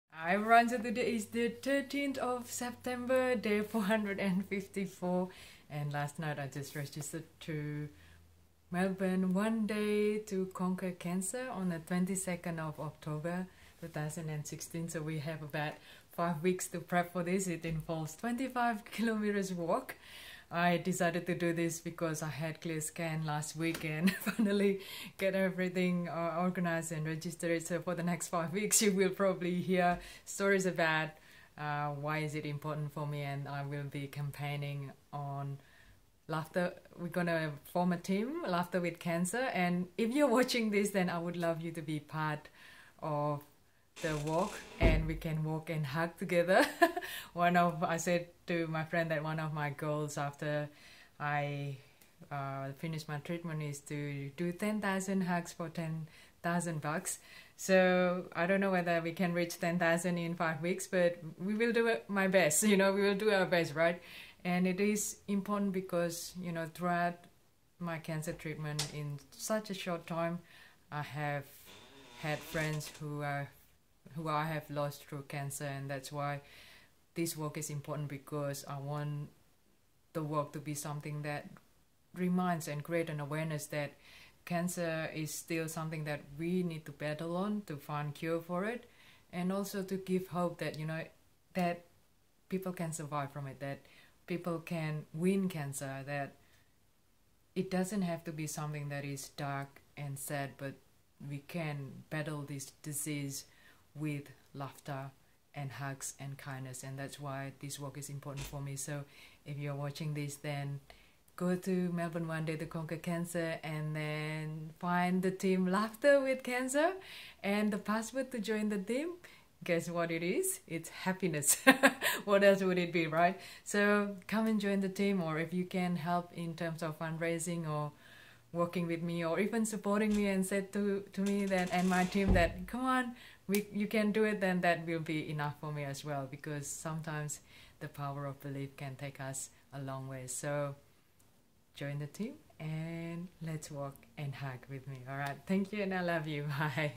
D454: ONEDAY TO CONQUER CANCER
Promised myself to do this if I get a clear scan to bring hope to my cancer friends and family